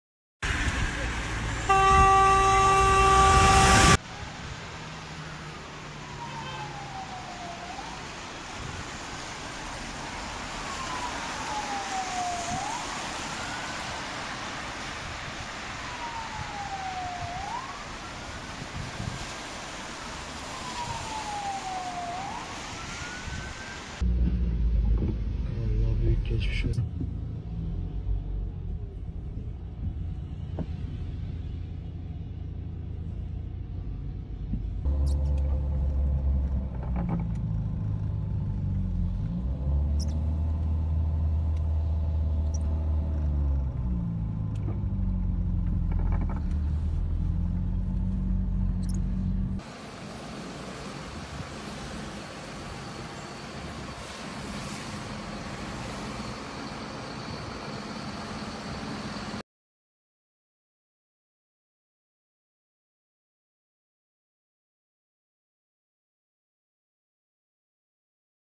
Adapazarı'nda ticari araç alev topuna döndü!
Sakarya’nın Adapazarı ilçesinde alev topuna dönen hafif ticari araçtan geriye hurda yığını kaldı.